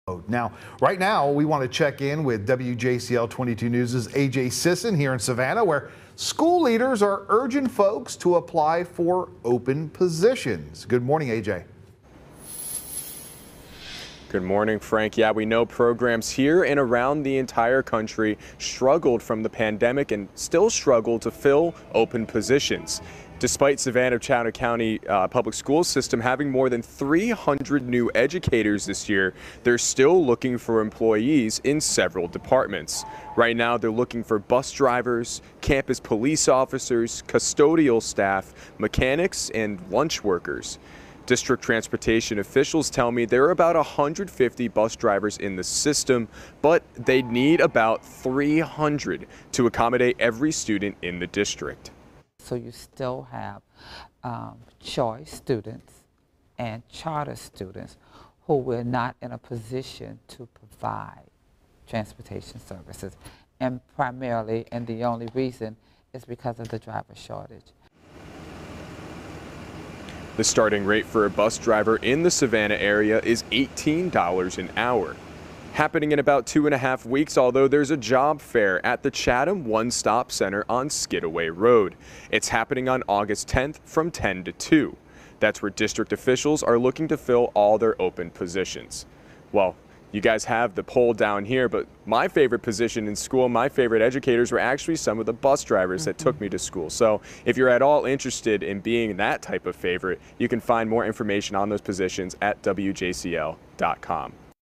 UPDATED SCCPSS looking for bus drivers and other positions
Despite hiring around 300 bus drivers, the Savannah Chatham County Public School System is still in need of bus drivers and other positions.